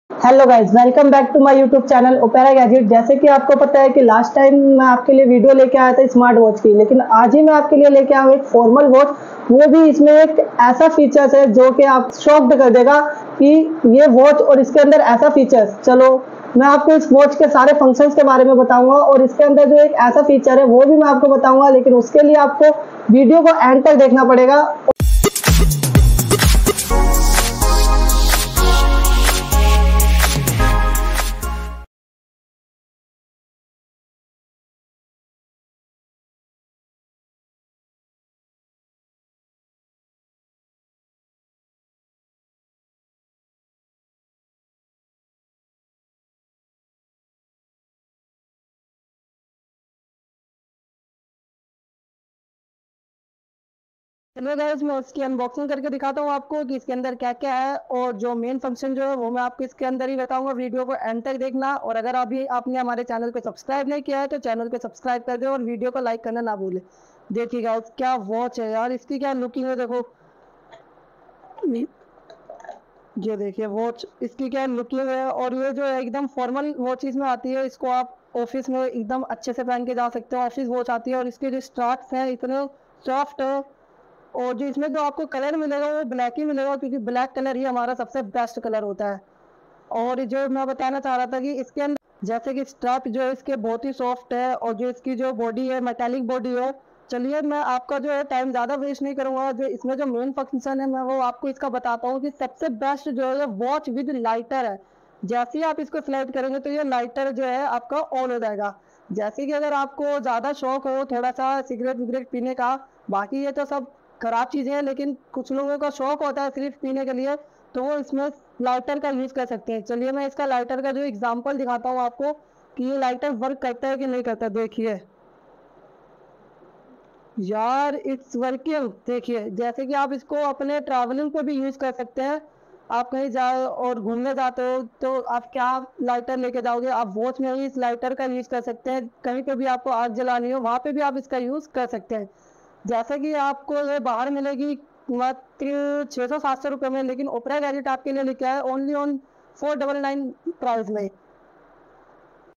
Cigarette Lighter Watch - The Ultimate Dual Function Accessory | cigarette lighter watch 2023
Looking for a unique and stylish way to light up your cigarette? Look no further than the cigarette lighter watch! This innovative product combines the convenience of a wristwatch with the functionality of a lighter, making it a must-have accessory for any smoker.In this video, we'll be giving a comprehensive review and unboxing of the cigarette lighter watch, which is currently available for sale at a price of 499 INR from Opera Gadget Store. However, if you prefer to purchase it online, you will need to pay shipping charges and half of the payment in advance.

One of the standout features of the cigarette lighter watch is its convenience. With this product, you'll always have a lighter on hand whenever you need it, without having to carry around a bulky lighter or fumble around with matches.
Overall, we highly recommend the cigarette lighter watch to any smoker who is looking for a convenient and stylish way to light up. So why wait? Head over to Opera Gadget Store or order online today and start enjoying the benefits of this fantastic product!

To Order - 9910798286